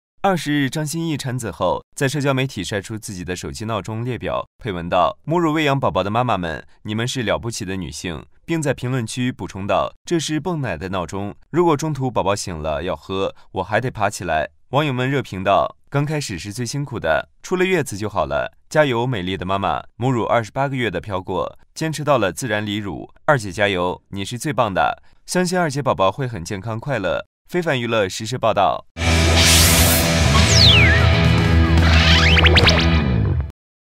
【2年前】張歆藝曬每日泵奶鬧鐘圖 感嘆做母親不易！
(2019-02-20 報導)

2月20日上午，張歆藝通過微博曬出一組圖片，圖片中是每日提醒她泵奶的鬧鐘設置，除了正常三餐外，凌晨零點、四點半也需要起來泵奶。同時，她還配文感嘆道：母乳餵養寶寶的媽媽們，你們是了不起的女性。